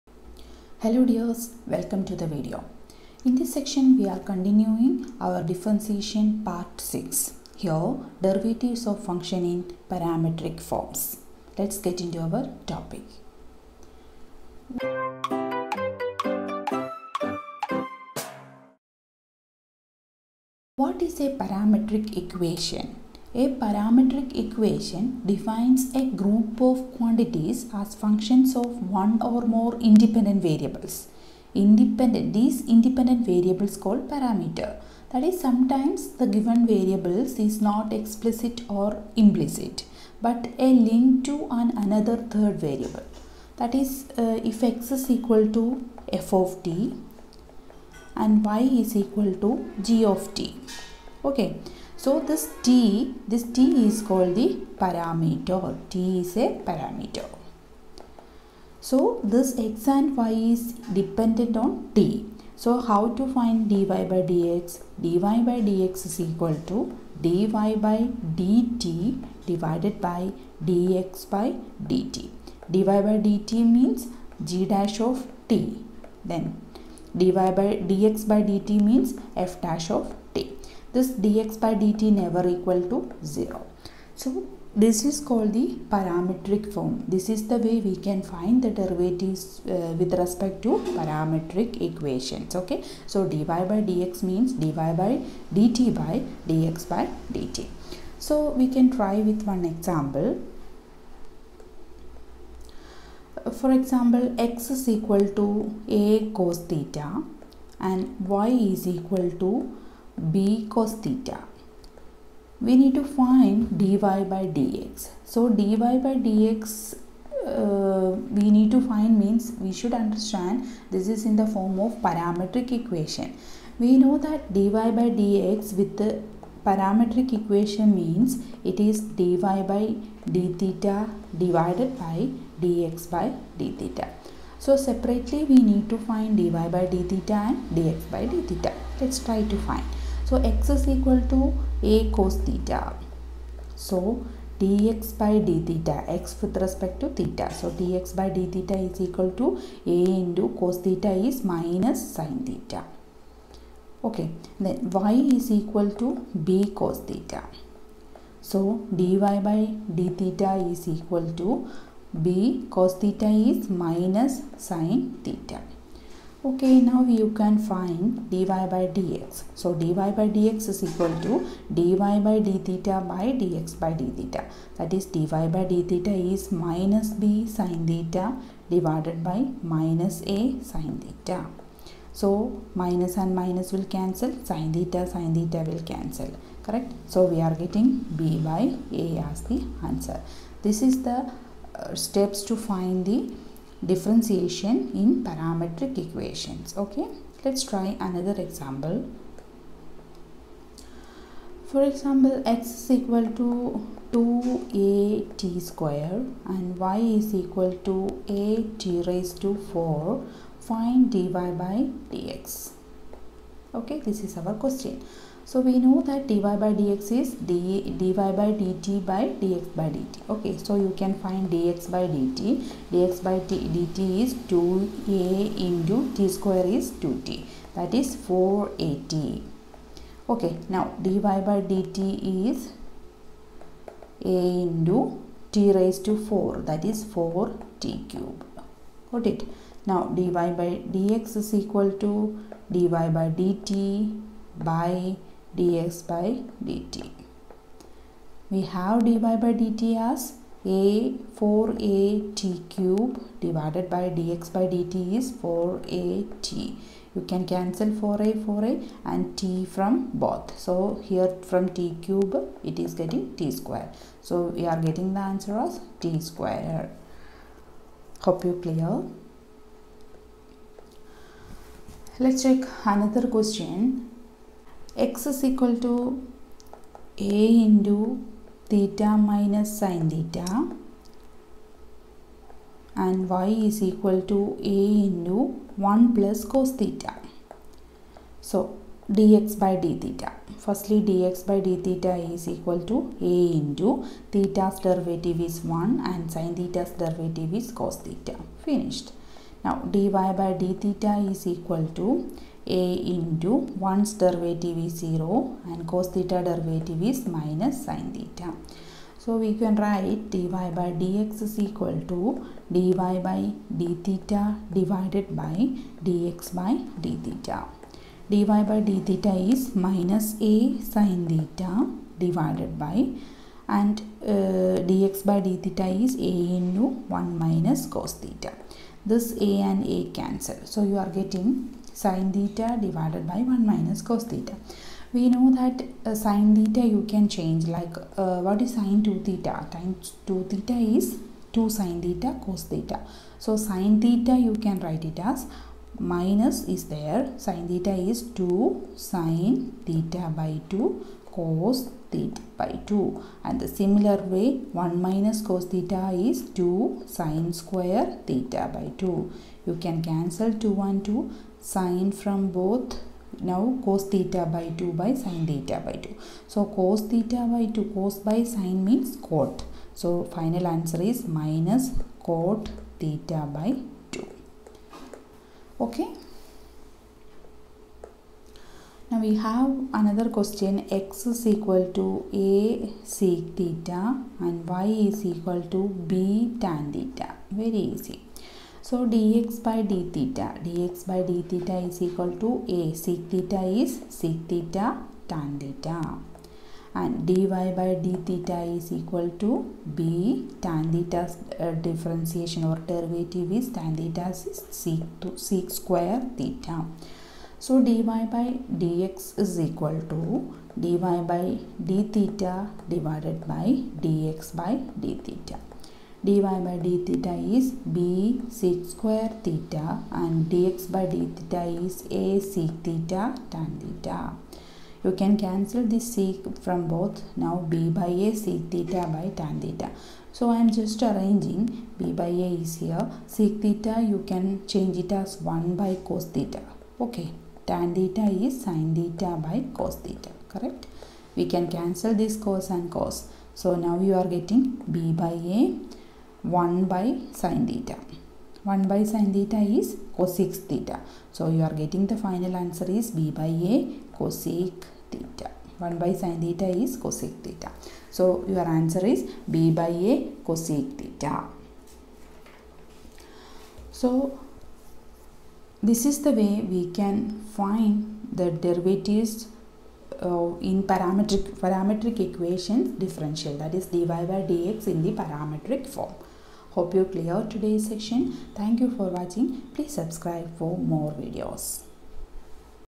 Differentiation part 6/derivatives of a function in a parametric form/Grade 12 MATH DIFFERENTIATION
Welcome to SS MATH SOLUTIONS 🙏😍😍
Video no. 135
Here discussing 👇👇
continuity and differentiability
diffferentiability grade 12
deeivatives of a function in parametric form
CBSE GRADE 12 MATHEMATICS DIFFERENTIATION

Thank you for coming here and studying with me 🙏🙏😍